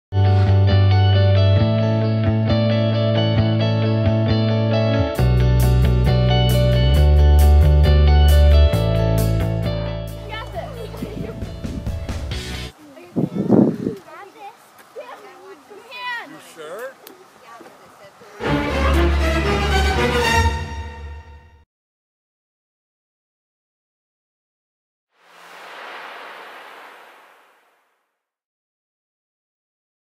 Tracknacy episode 1: THE FALL. Tracknacy episodio 1: LA CAÍDA
Brooklyn (Boston) and Coralline (Baxtor) are on the monkey bars there trying to get to the other side but Brooklyn (Boston) lost her grip and grabed Coralline's (Baxtor) hand and she fell too. Then they went some ware.  (C)  2016 D. L. Crazy kid studios.   Brooklyn (Boston) y coralinas (Baxtor) están en las barras allí tratando de llegar al otro lado, pero Brooklyn (Boston) perdieron su agarre y grabed la mano de coralina (Baxtor) y cayó también. Luego se fueron un poco de mercancías. Derechos de autor. 2016 cabrito estudios D. L. locas.    English. Horror. Need to be over 11 years old unless you are a chicken. Inglés. Horror. Necesidad de ser más de 11 años de edad a menos que seas un pollo